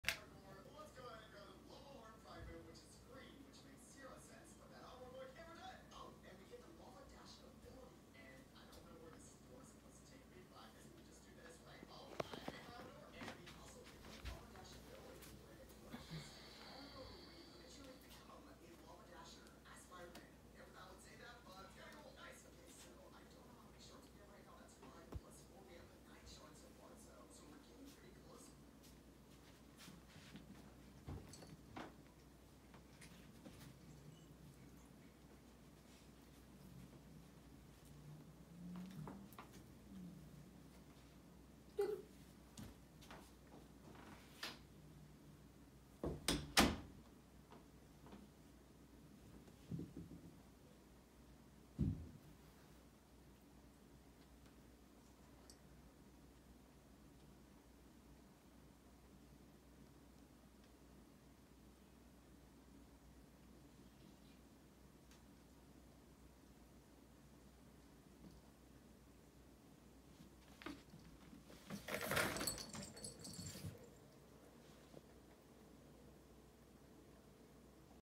Luna (black and white) is looking for her forever home!
Meet Luna! She’s an adorable tuxedo and such a sweet girl! She loves to play with toys and in her tunnel. She’s friendly and likes to cuddle. She definitely needs a kitty buddy to play with. She wanted to be friends with the older resident cat in her foster home and he finally gave in to her! She would be OK with a more calm cat-friendly dog as long as she has a cat friend, too. She can play a little rough for younger kids, but is fine with the foster’s son who is 9. Luna is litter box trained. She is eating wet and dry food. Luna is spayed, vaccinated, microchipped and FIV/FeLV negative. Her adoption fee is $150. She is being fostered in Denver. If you are interested in adopting Luna, please submit an application